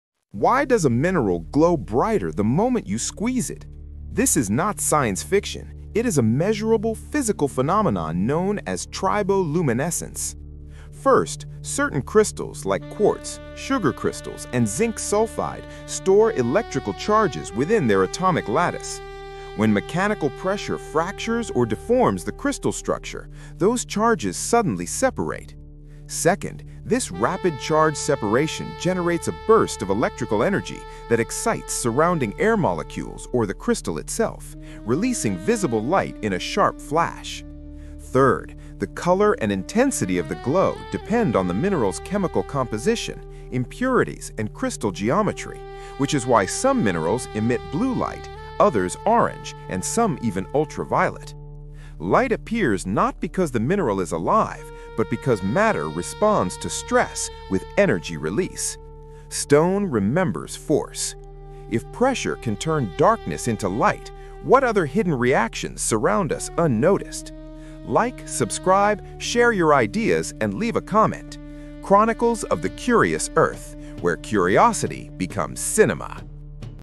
Why Does This Mineral Glow Brighter When You Squeeze It?
Why do some minerals emit light when squeezed or crushed? This video explains triboluminescence, a real physical phenomenon where mechanical stress causes electrical discharge and visible light emission in certain crystals.

Scientific Sources:
– Encyclopaedia Britannica: Triboluminescence

– Physics Today: Light Emission from Mechanical Stress in Crystals